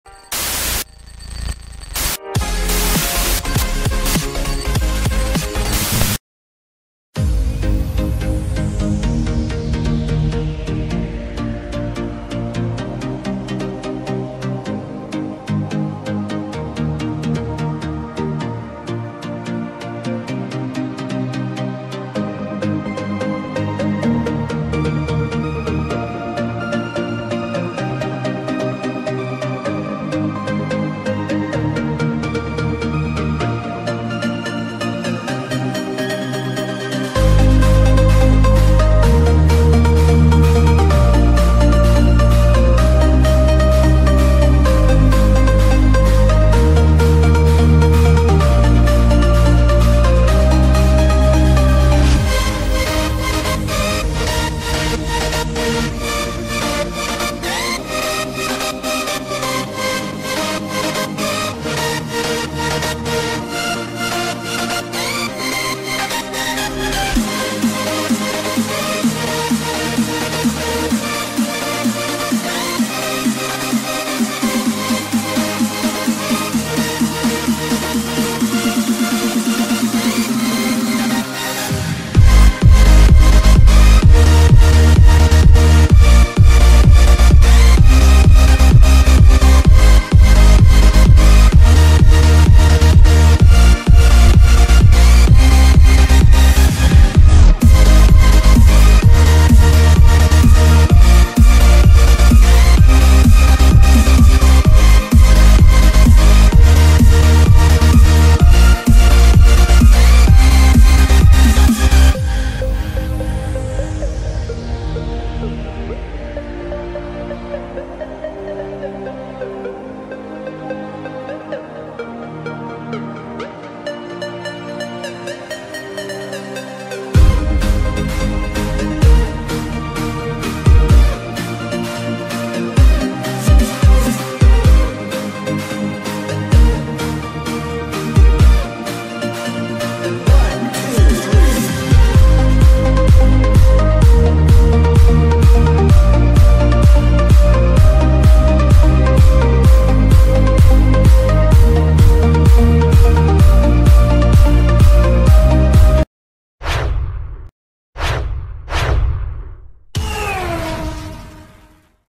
Baga Beach Goa Nightlife | Full Of Lights, Decoration & Disco
Baga Beach Goa is one of the finest beach situated in the North Goa. Lots of visitors visits to this beach every day. So it's publicity is increasing day by day. Therefore, our team also tried to show some night views from the Baga Beach Goa.